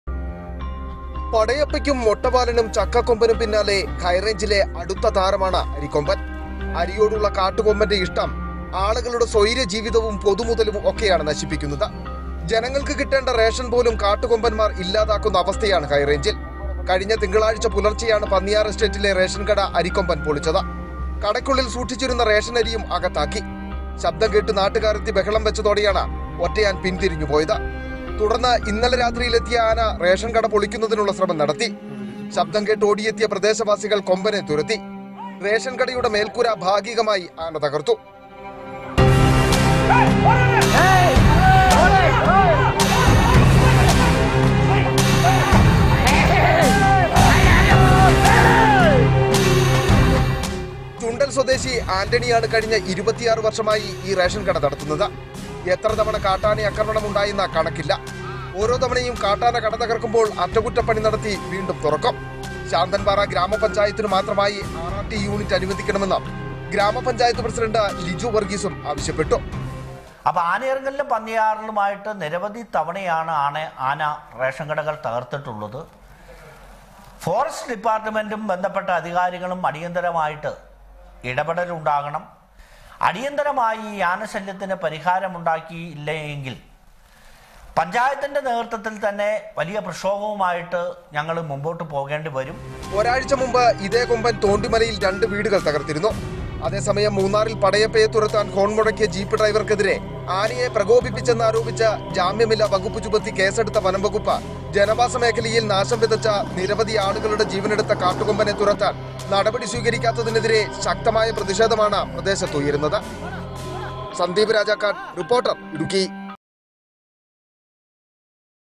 ഇടുക്കി: റേഷൻ കടയിൽ വീണ്ടും അരിക്കൊമ്പൻ എന്ന കാട്ടാനയുടെ ആക്രമണം
ഇടുക്കി ശാന്തൻപാറ പന്നിയാർ എസ്റ്റേറ്റിലെ റേഷൻ കടയിൽ വീണ്ടും അരിക്കൊമ്പൻ എന്ന കാട്ടാനയുടെ ആക്രമണം. റേഷൻ കടയുടെ മേൽക്കൂര ഭാഗികമായി തകർത്തു. നാട്ടുകാർ ബഹളം വെച്ച് ആനയെ തുരത്തി. പത്തോളം പേരുടെ ജീവനെടുത്ത കാട്ടാനയാണ് അരിക്കൊമ്പൻ.

Shows Telecasted:

* Editor's Hour
* Meet The Editors
* Close Encounter
* 3 PM Debate
* Reporter Interactives
* My Doctor

With Regards
Team Reporter